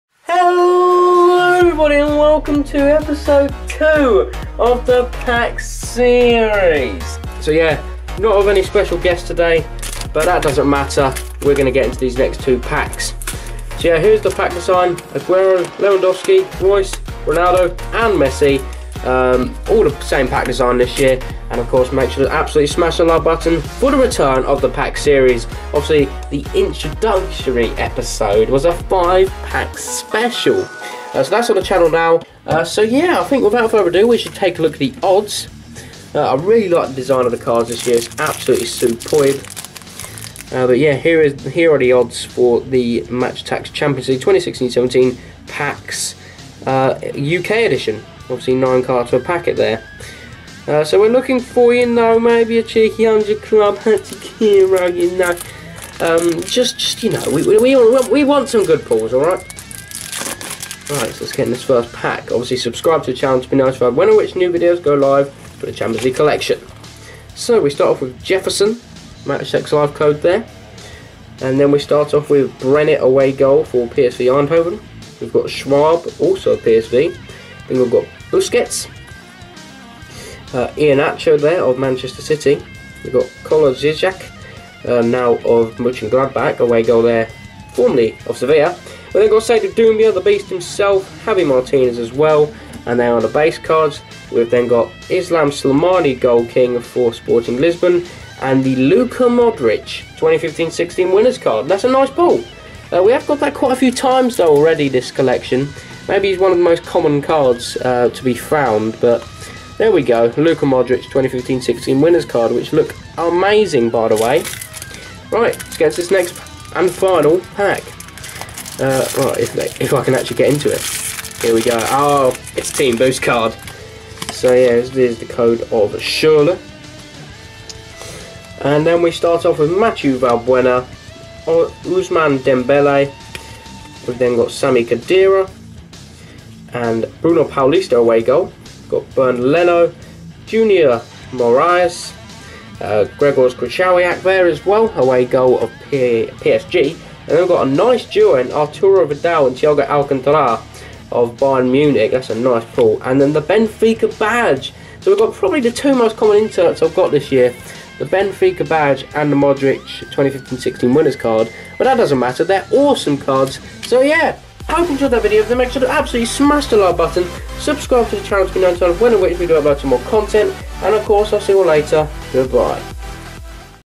ERROR CARD??!! | MATCH ATTAX CHAMPIONS LEAGUE 2017 | PACKS #2
We pull a *kind of* error card in the second episode! Find out who it is in this opening!

☆If you really want, SHARE the video. It all helps out massively!

☆Let me know if you are going to go to the Swap and Play tours!☆